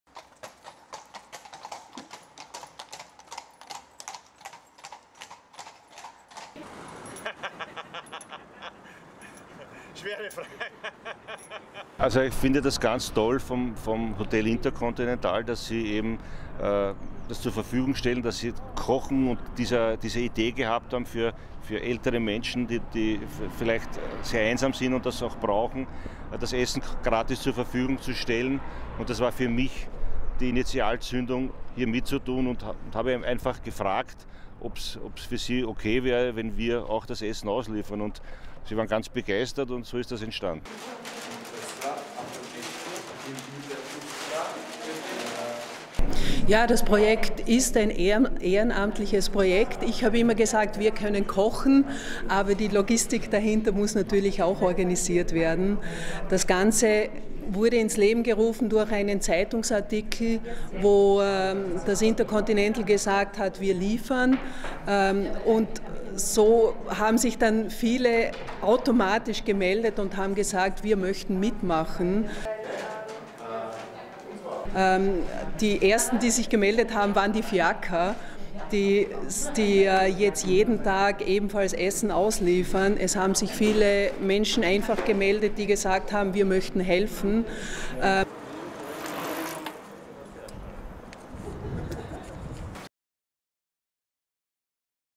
旅游业停滞期间 维也纳观光马车提供送餐服务
因新冠疫情而导致旅游业停滞期间，维也纳的标志性观光马车也被迫陷入了停业的处境。一些车夫开始利用马车歇业的空档期，为当地的老人提供送餐服务。